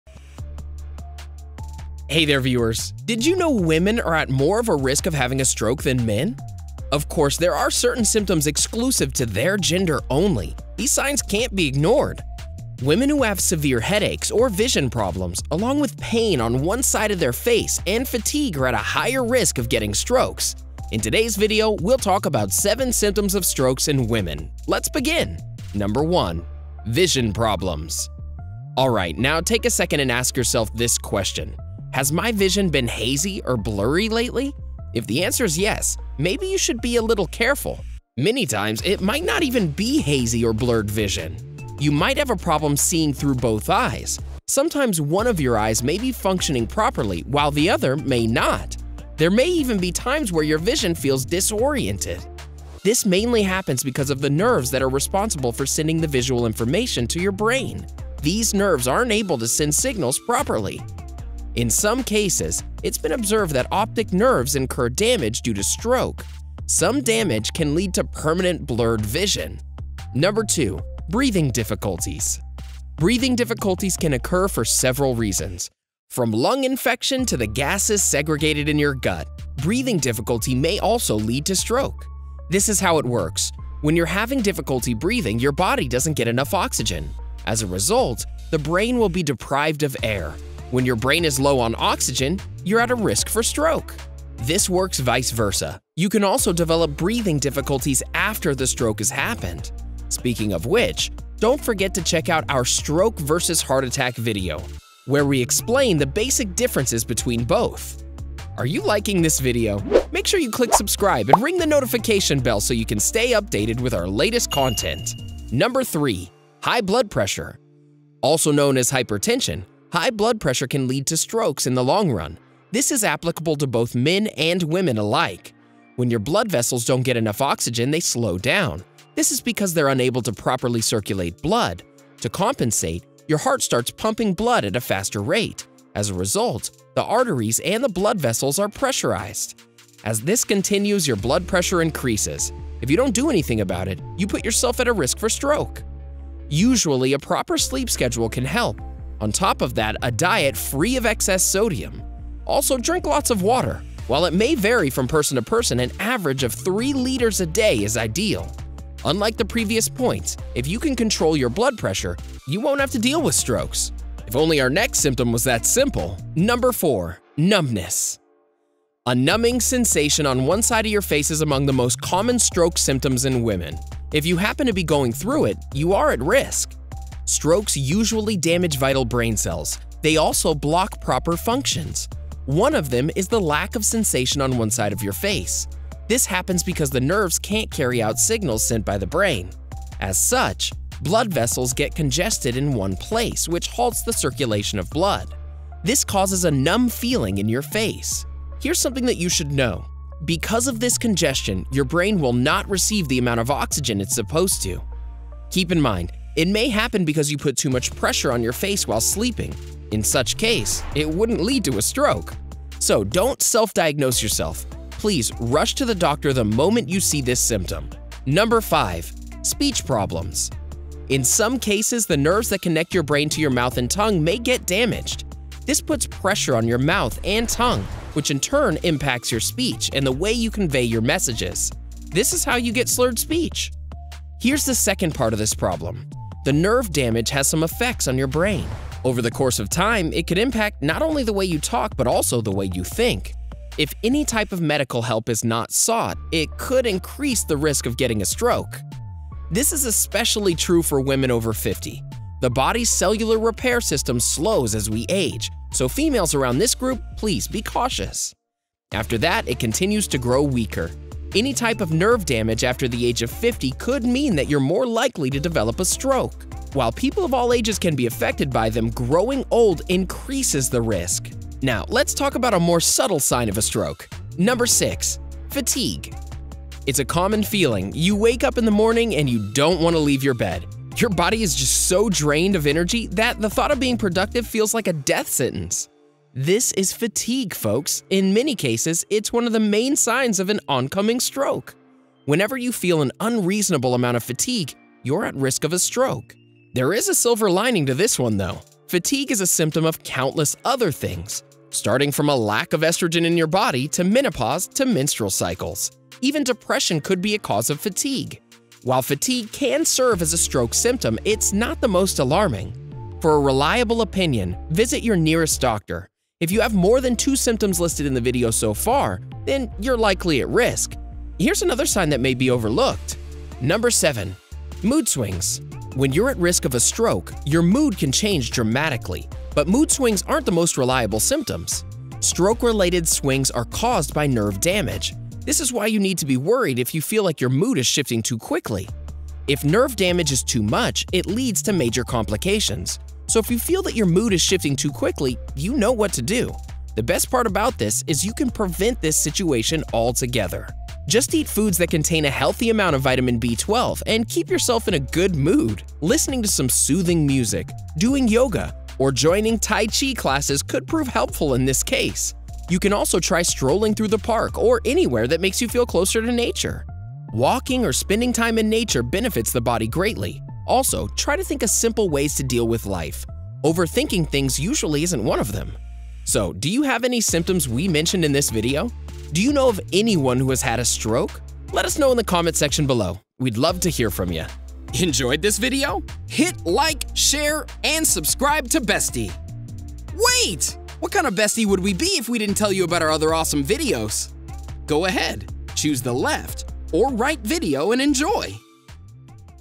7 Symptoms Of Strokes In Women
"Women who have severe headaches or vision problems, along with pain on one side of your face and fatigue are at a higher risk of getting strokes. In this video, we will talk about 7 symptoms of strokes in women. Let’s begin.

Timestamps:
Intro - 0:00
1. Vision Problems - 00:26
2. Breathing Difficulties - 01:10
3. High Blood Pressure - 01:56
4. Numbness - 02:47
5. Speech Problems - 03:38
6. Fatigue - 04:35
7. Mood Swings - 05:24

Summary:
1. Vision Problems
Alright now take a second and ask yourself this question, “Has my vision been hazy or blurred lately?” If the answer is yes, maybe you should be a little careful. Many times, it might not even be hazy or blurred vision. You might have problems seeing through both eyes. Sometimes one of your eyes may be functioning properly, but the other one may not. There will even be times where your vision feels disoriented.

2. Breathing Difficulties
Breathing difficulties can occur for several reasons. From a lung infection to the gases segregated in your gut. Breathing difficulty may also lead to stroke.

3. High Blood Pressure
Also known as hypertension, high blood pressure can lead to strokes in the long run. This is applicable to both men and women alike.

4. Numbness
A numbing sensation on one side of the face is among the most common stroke symptoms in women. If you happen to be going through it, you are at risk.

5. Speech Problems
In some cases, the nerves that connect your brain to your mouth and tongue may get damaged. This puts pressure on your mouth and tongue, which in turn impacts your speech and the way you convey your messages. This is how you get a slurred speech.